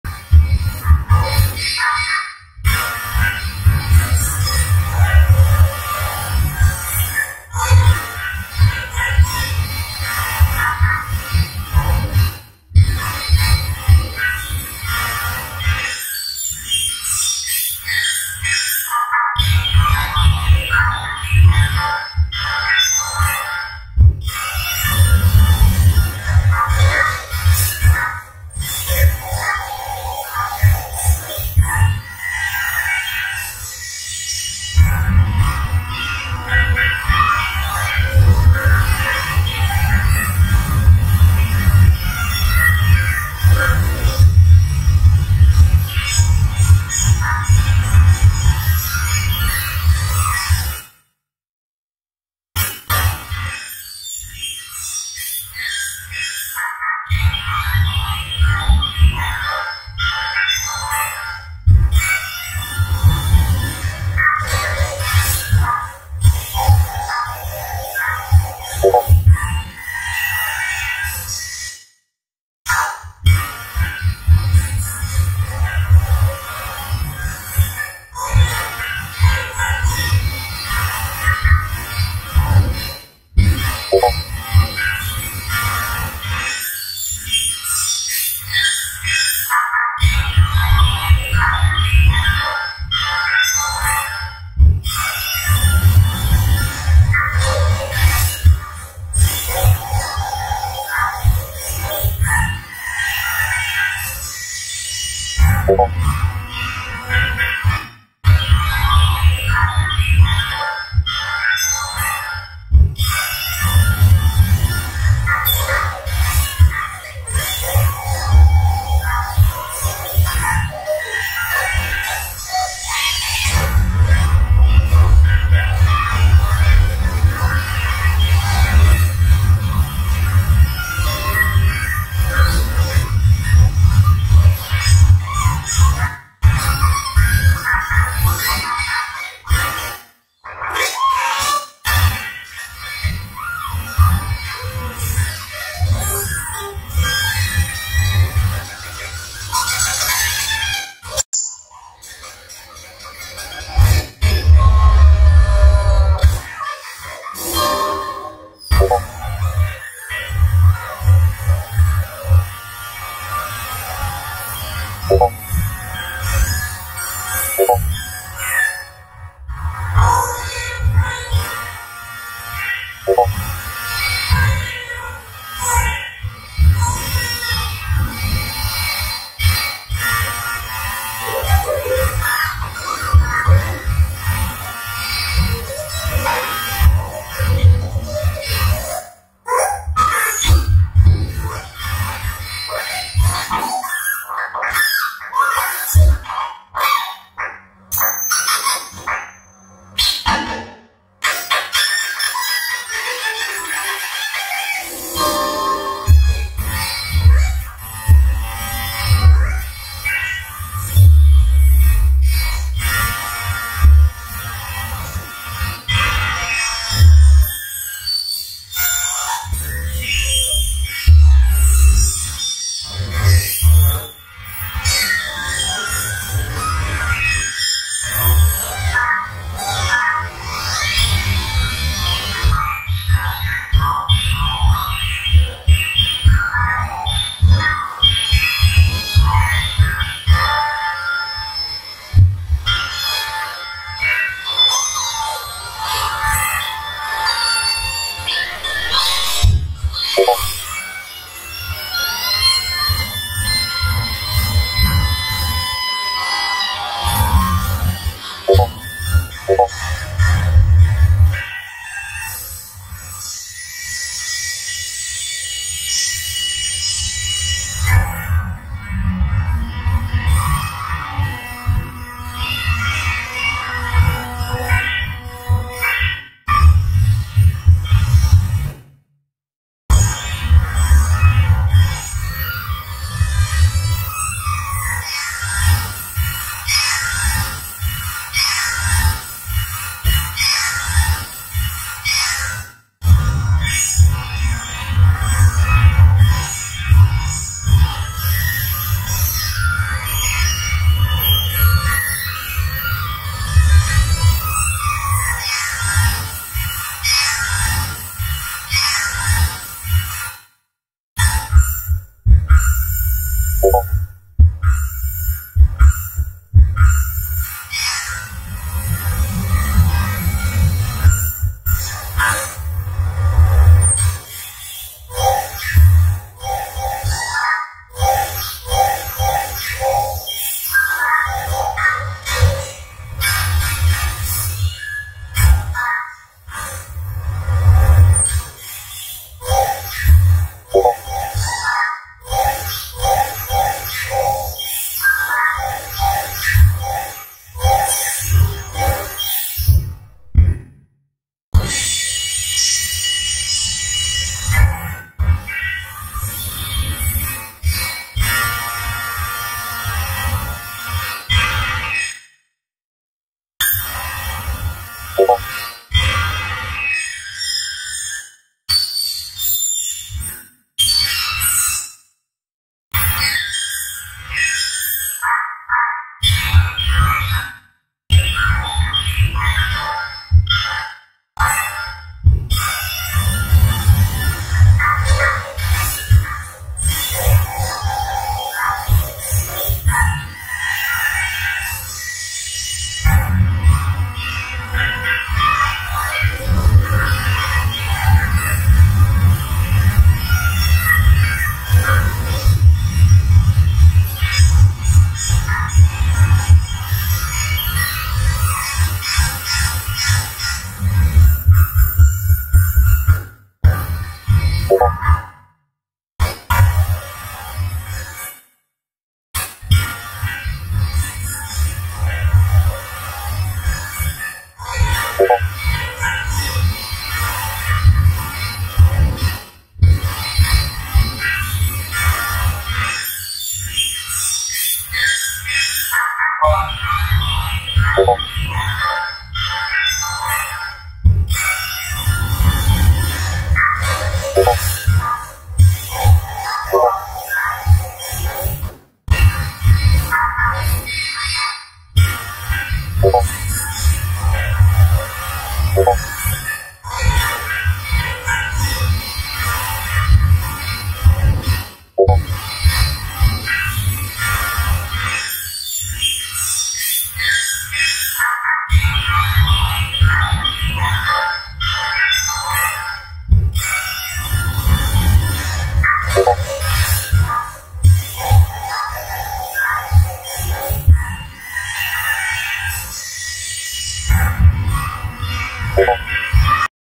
After the cancellation of The Bad Piggies Show
Ross decided to eat the Disguise Gem and morphs into anything!

Goodbye, @thebadpiggiesreturn.